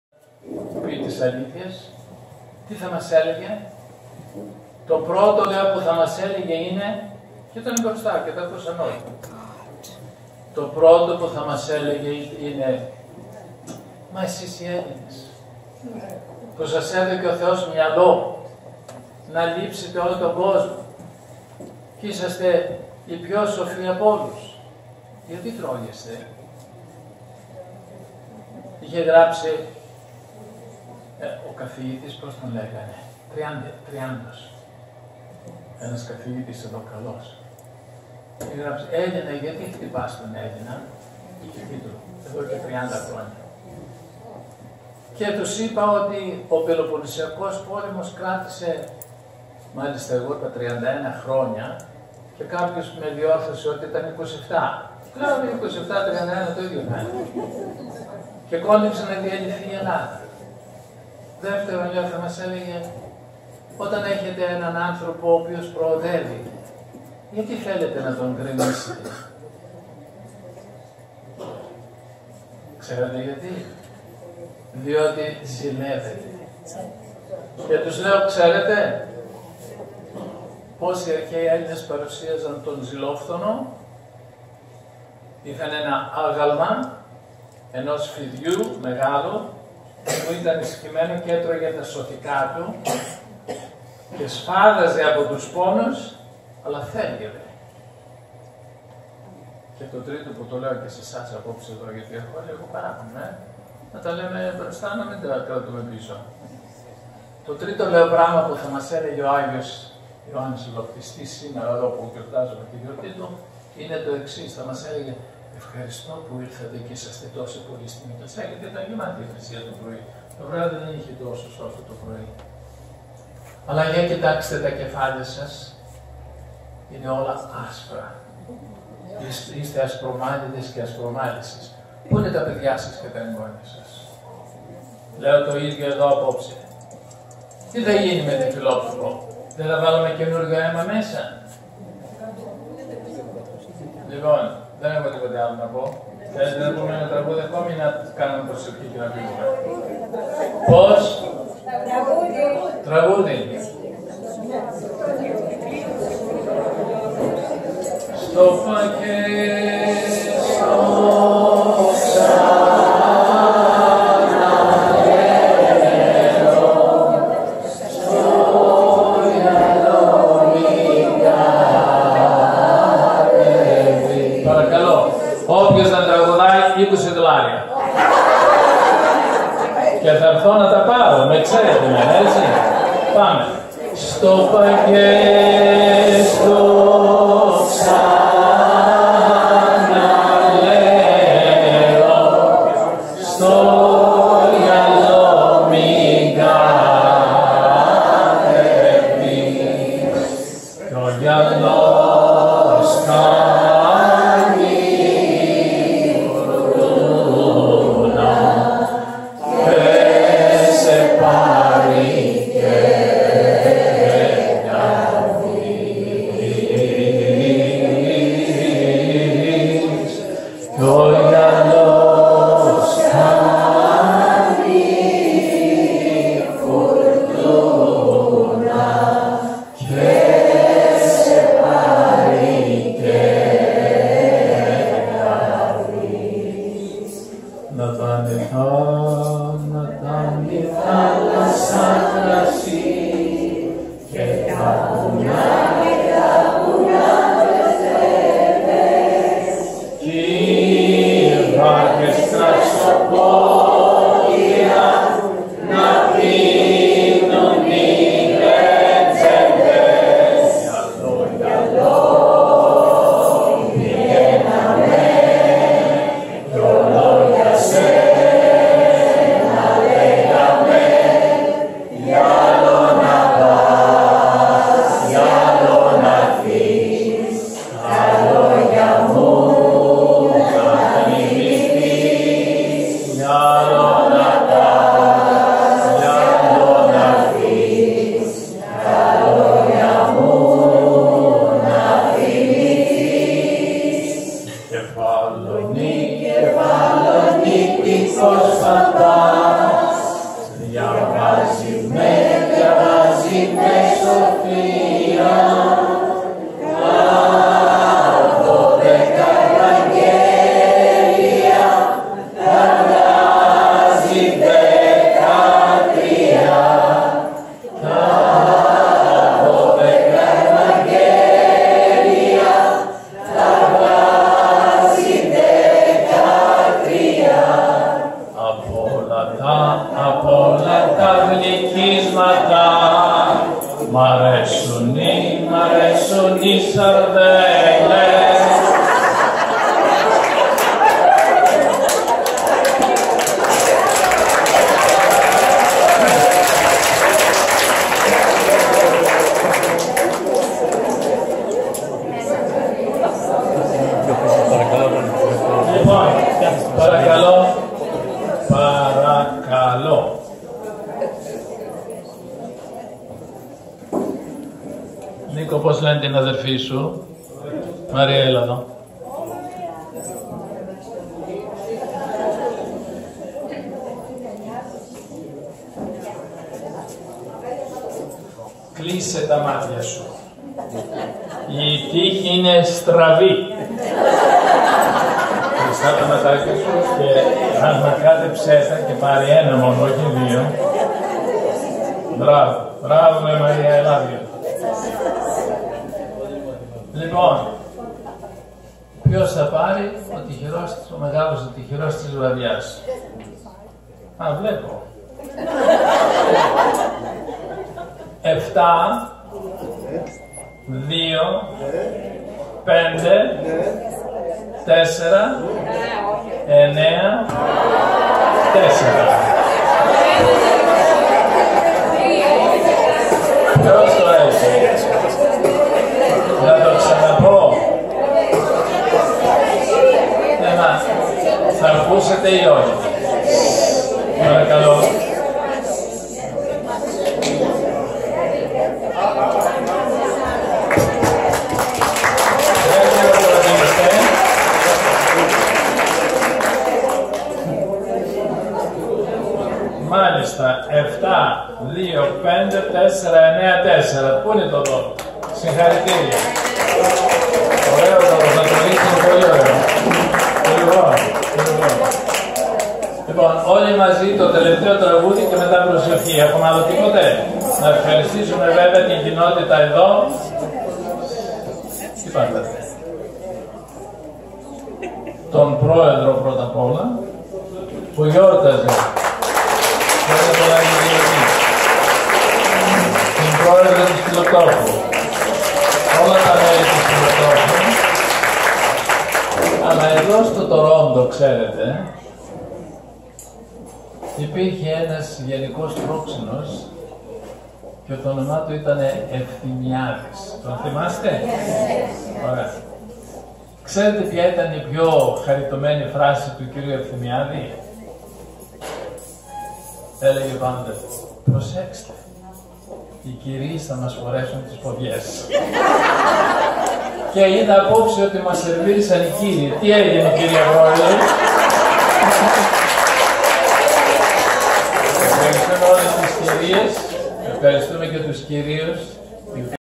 METROPOLIS PHILOPTOCHOS VASILOPITA DINNER - 25 - 01- 2018(2)
VASILOPITA DINNER HELD AT GREEK ORTHODOX CHURCH OF SAINTS CONSTANTINE & HELEN - TORONTO - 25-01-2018